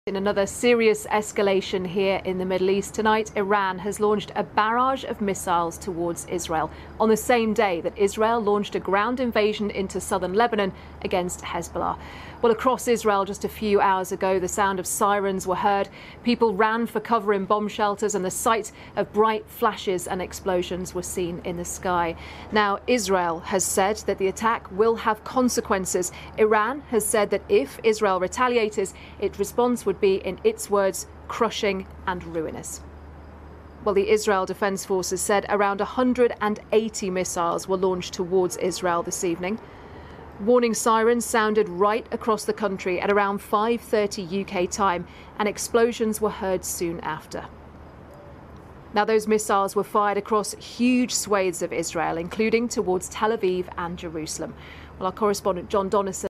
Iran FIRES MISSILES! How Will This Impact the GLOBAL OIL MARKET?
Iran’s launch of  missiles has sparked concerns over the stability of the global oil market. Will this military action disrupt oil supplies and cause prices to surge? Watch as we examine the potential economic fallout and the risks to global energy supplies in the wake of this missile strike.